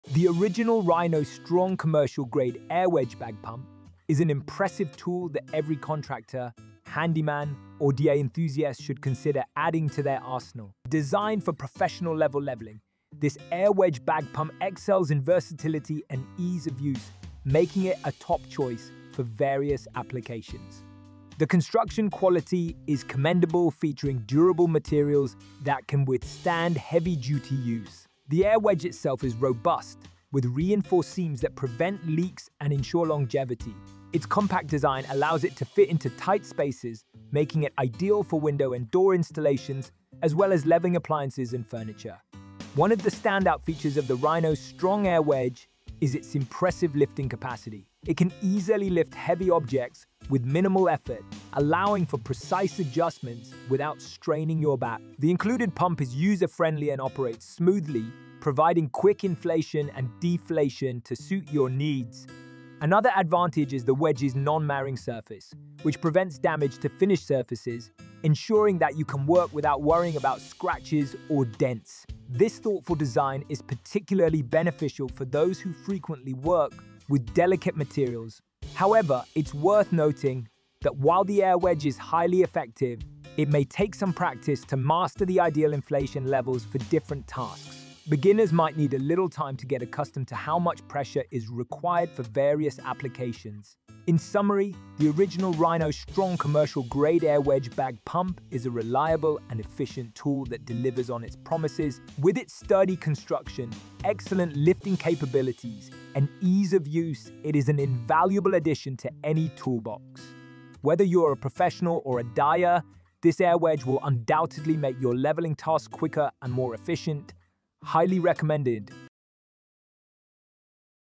The Original Rhino Strong Commercial Grade Air Wedge Bag Pump Professional Leveling Review
The Original Rhino Strong Commercial Grade Air Wedge Bag Pump is an impressive tool that every contractor, handyman, or DIY enthusiast should consider adding to their arsenal. Designed for professional-level leveling, this air wedge bag pump excels in versatility and ease of use, making it a top choice for various applications.

The construction quality is commendable, featuring durable materials that can withstand heavy-duty use. The air wedge itself is robust, with reinforced seams that prevent leaks and ensure longevity. Its compact design allows it to fit into tight spaces, making it ideal for window and door installations, as well as leveling appliances and furniture.

One of the standout features of the Rhino Strong air wedge is its impressive lifting capacity. It can easily lift heavy objects with minimal effort, allowing for precise adjustments without straining your back. The included pump is user-friendly and operates smoothly, providing quick inflation and deflation to suit your needs.

Another advantage is the wedge's non-marring surface, which prevents damage to finished surfaces, ensuring that you can work without worrying about scratches or dents. This thoughtful design is particularly beneficial for those who frequently work with delicate materials.

However, it’s worth noting that while the air wedge is highly effective, it may take some practice to master the ideal inflation levels for different tasks. Beginners might need a little time to get accustomed to how much pressure is required for various applications.

In summary, the Original Rhino Strong Commercial Grade Air Wedge Bag Pump is a reliable and efficient tool that delivers on its promises. With its sturdy construction, excellent lifting capabilities, and ease of use, it is an invaluable addition to any toolbox. Whether you're a professional or a DIYer, this air wedge will undoubtedly make your leveling tasks quicker and more efficient. Highly recommended!